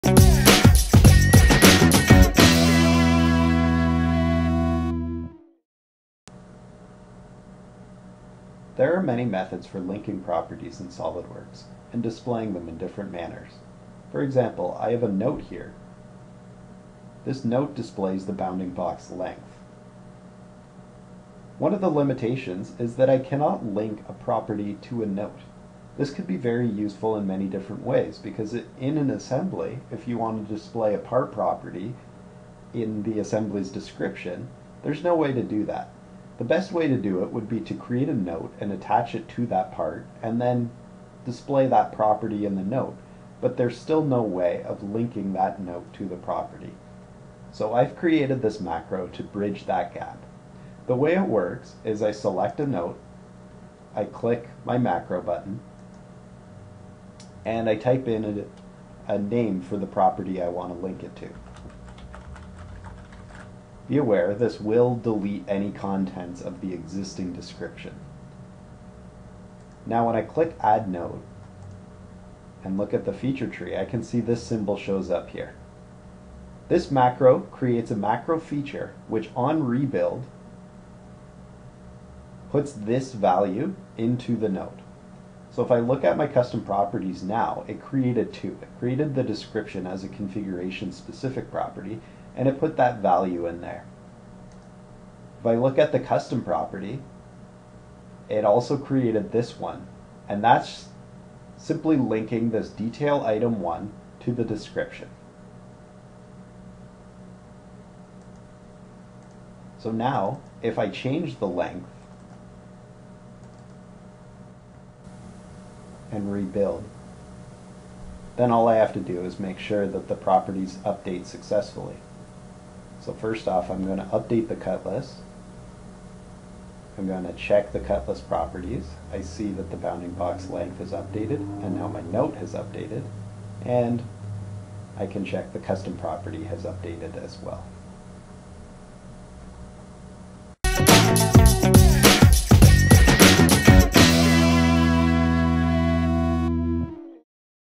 How to link SOLIDWORKS Custom Properties to Notes
A great way for linking cut-list properties to custom properties at part level.